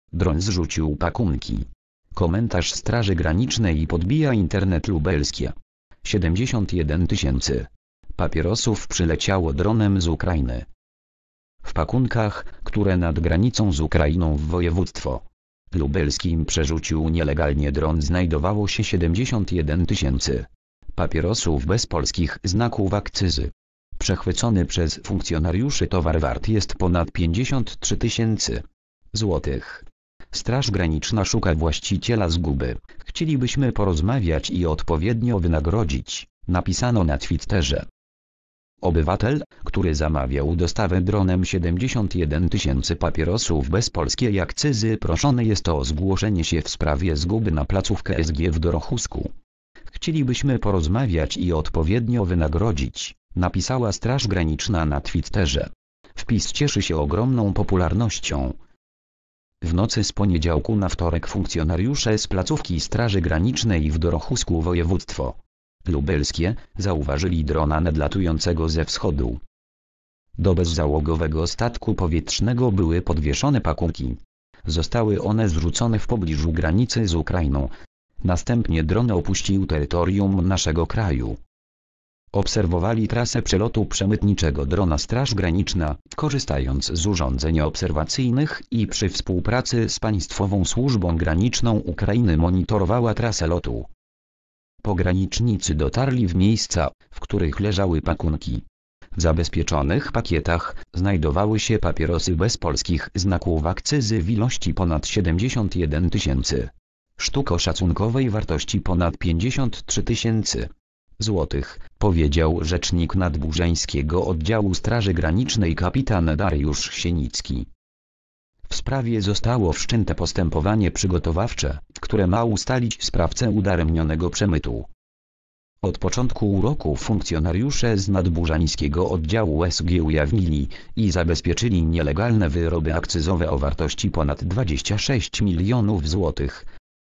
Dron zrzucił pakunki. Komentarz Straży Granicznej podbija internet.
Lubelskie. 71 tys. papierosów przyleciało dronem z Ukrainy.

  (fot. Straż Graniczna /materiały prasowe)